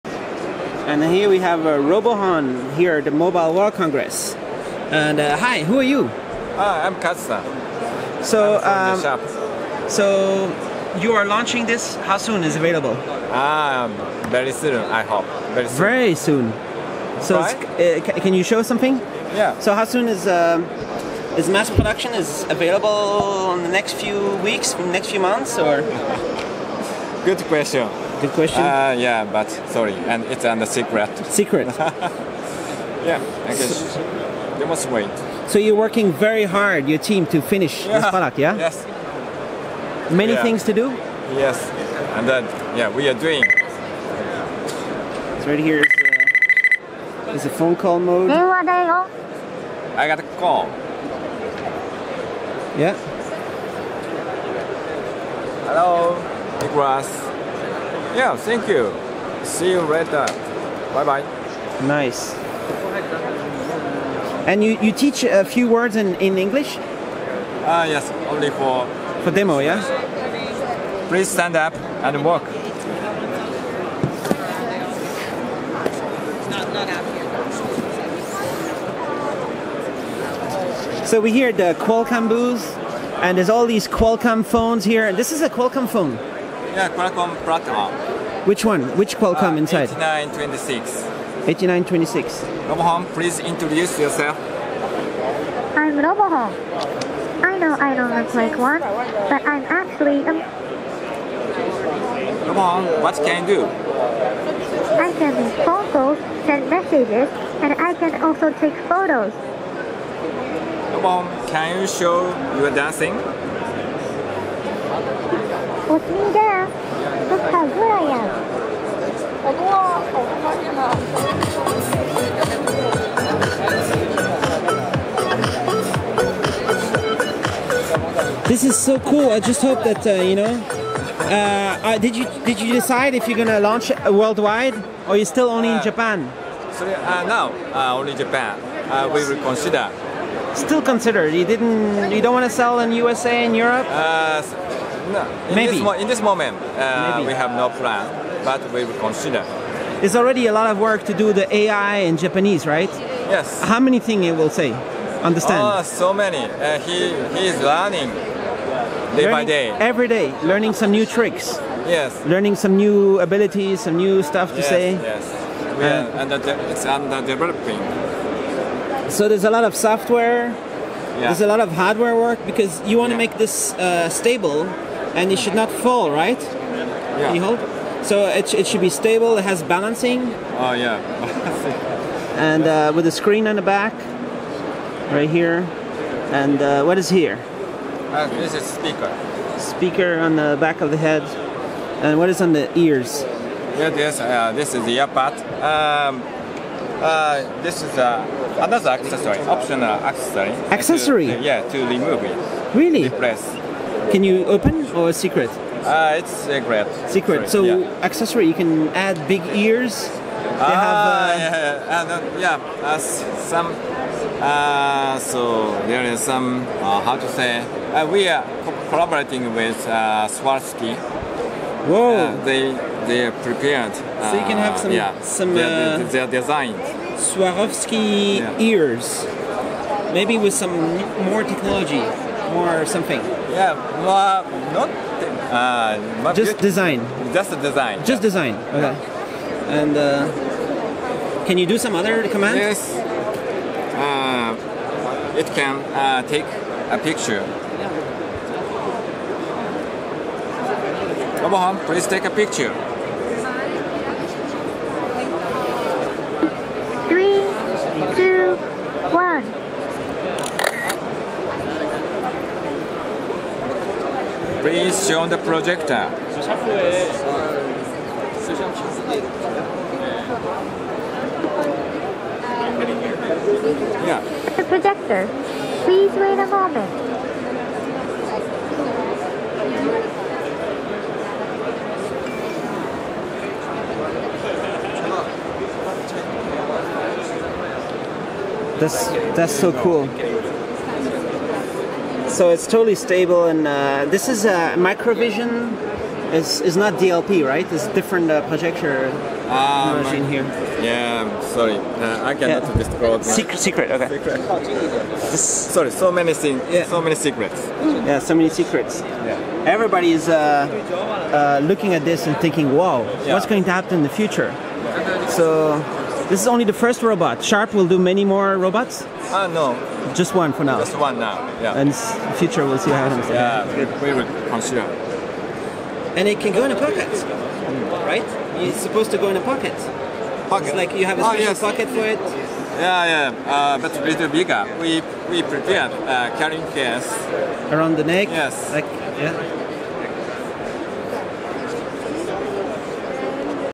Sharp Robohon speaks English at the Qualcomm booth at MWC 2016
is Sharp's vision for the future of the smartphone. A pocket-sized humanoid Robot, with built-in Qualcomm Snapdragon 400 processor, LTE Sim card slot, it has a built-in projector, microphone, speaker. The concept is to use AI conversations to bring usefulness as a notification robot. I think Sharp needs to mass manufacture it, sell it worldwide for the price of an iPhone, and load all kinds of AI apps so it can be powered by Google, Amazon, Microsoft or anyone else who is making an AI voice assistant.